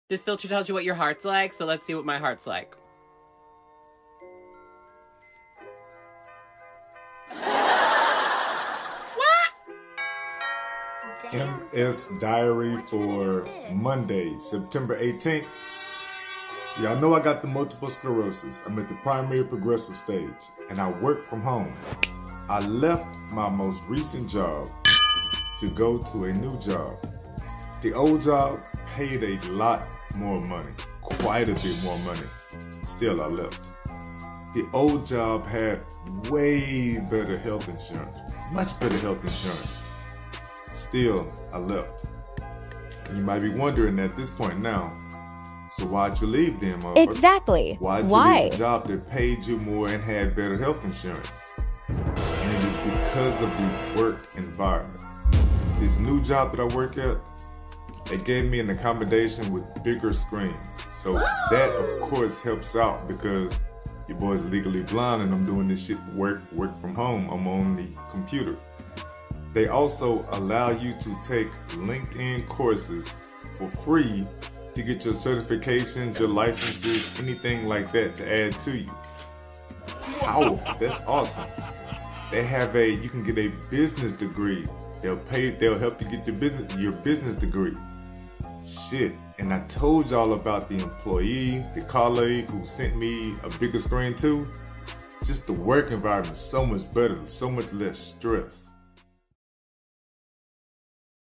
MS Diary | Monday, 9/18/23 | Why I'm leaving better pay and health insurance
Wow. A lot has changed. I uses to think more money was the solution. Then, Primary Progressive Multiple Sclerosis so I thought better healthcare.

Now? I'm going better work environment.